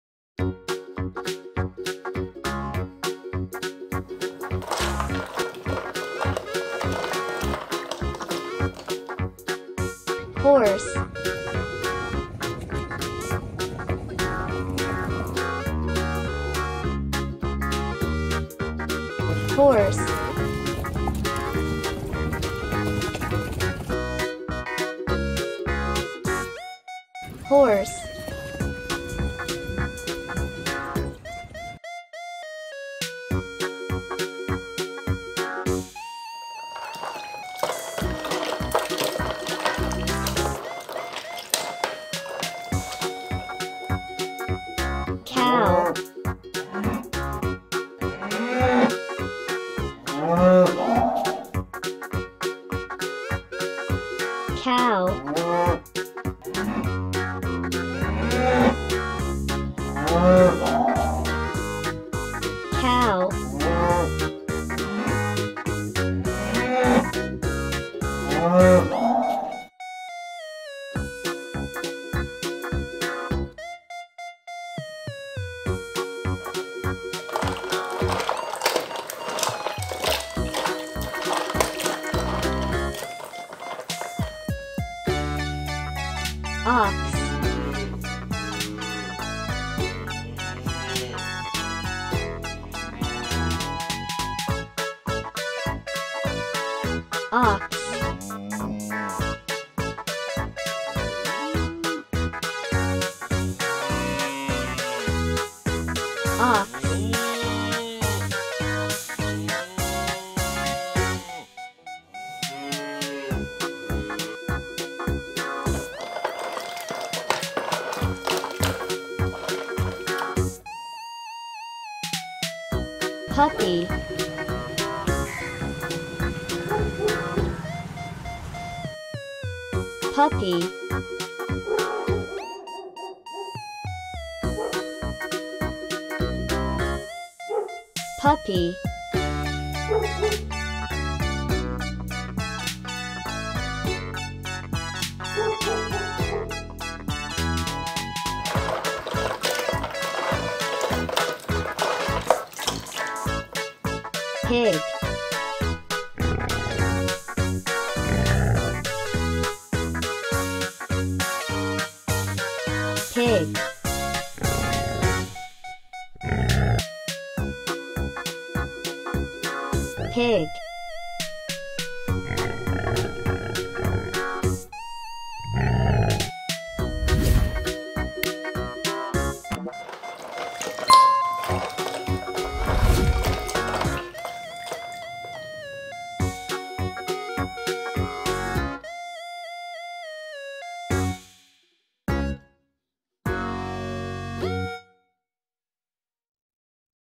Aprenda Nomes de Animais | Porco, Vaca, Cachorro. Desenhos Educativos para Crianças
- PORTUGUÊS - ENGLISH
- CAVALO         - HORSE
- VACA              - COW
- BOI                  - OX
- CACHORRO   - PUPPY
- PORCO           - PIG